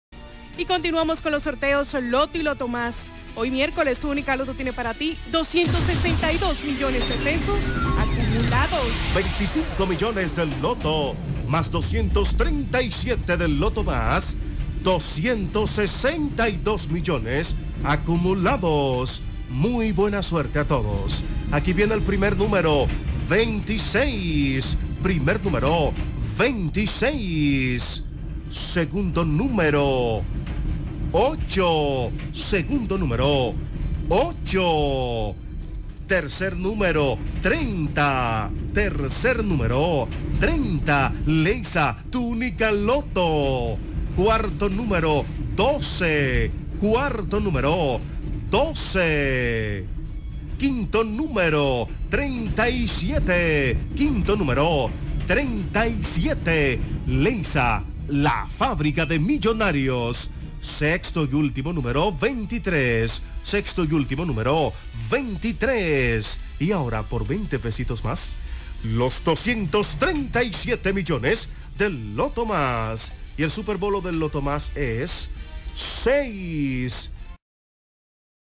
Resultados del loto y loto más de LEIDSA - Miércoles 13 de febrero 2019
Resultados del Loto y Loto más de LEIDSA para este miércoles 13 de febrero 2019

Nota: NO SOMOS LEIDSA